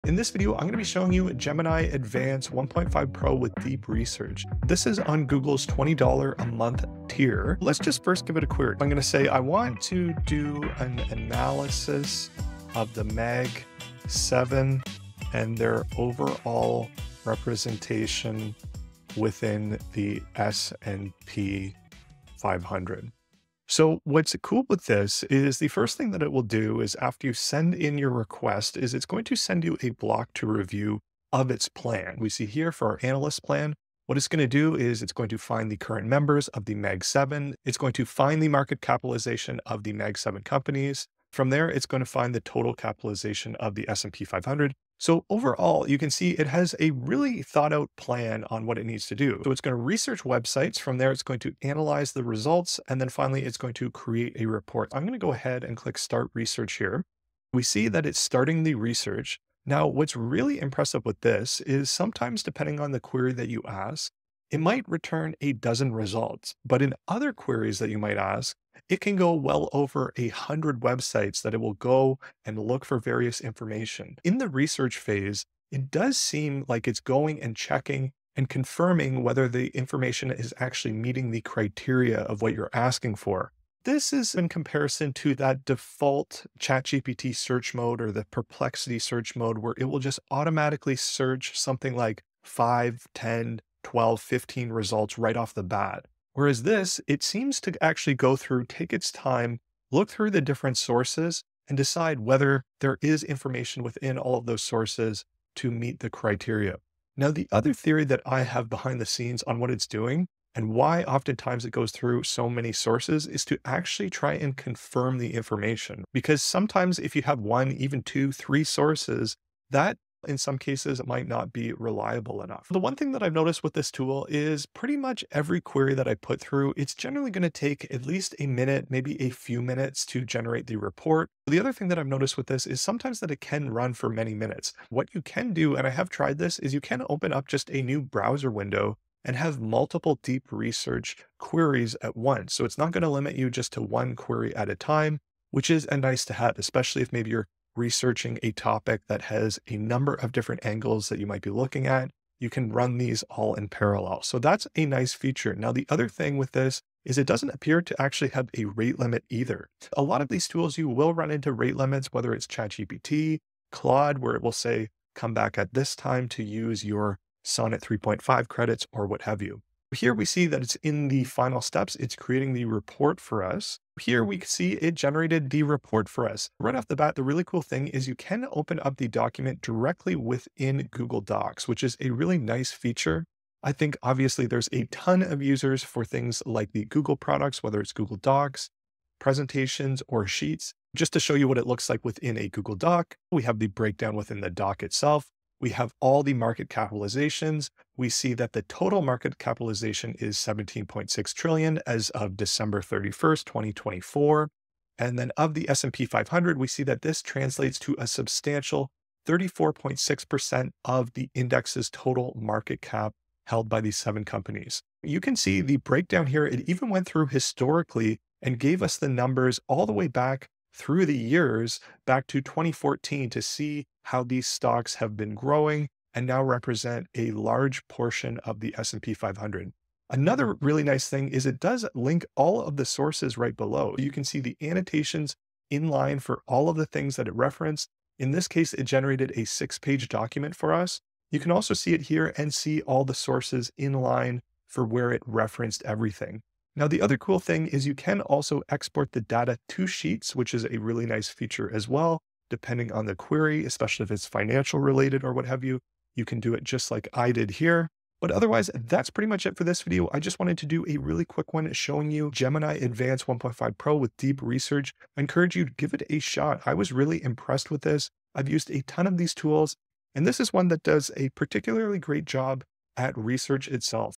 Gemini Deep Research in 4 Minutes
Deep Dive into Gemini Advanced 1.5 Pro: Google’s Powerful Research Tool!

Learn The Fundamentals Of Becoming An AI Engineer On Scrimba;

In this video, I showcase the capabilities of Gemini Advanced 1.5 Pro with deep research on Google's $20/month tier. I test the tool by analyzing the 'Magnificent Seven' companies and their representation in the S&P 500. The tool provides a detailed research plan, examines numerous websites, and thoroughly verifies information before generating a comprehensive report. Furthermore, I demonstrate how it seamlessly integrates with Google Docs, offering detailed breakdowns, historical data, and source annotations. You can also export data to Google Sheets. This tool impresses with its thoroughness and efficiency, allowing multiple, parallel deep research queries without rate limits.

00:00 Introduction to Gemini Advanced 1.5 Pro
00:07 Setting Up the Analysis Query
00:54 Research Process and Methodology
02:49 Report Generation and Features
04:06 Conclusion and Final Thoughts